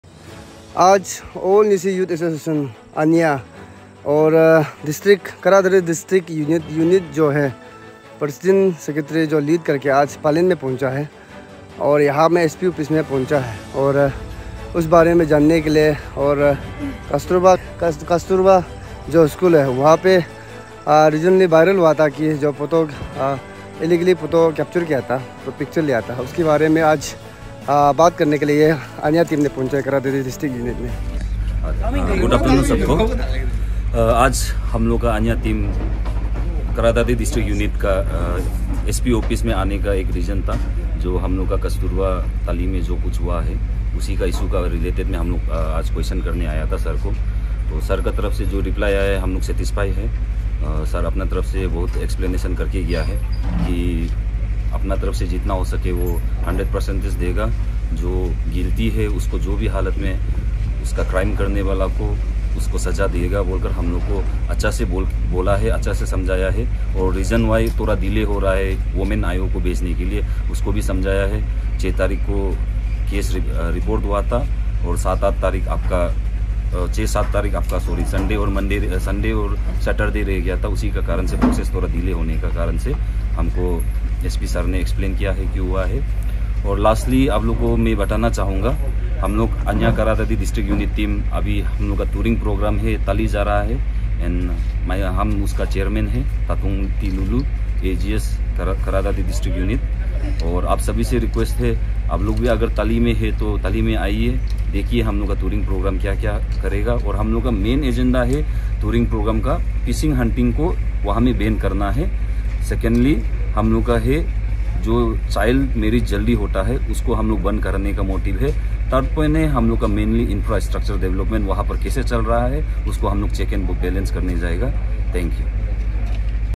Today 9/09/2024 ANYA Kra Daadi district unit executive visit SP office
Today 9/09/2024 ANYA Kra Daadi district unit executive visit SP office kra daadi to discuss regarding the recent issue of Kasturba Gandhi school. A fir was lodged on 6th September at Palin Police station..to keep law and order and to aware public on this issue.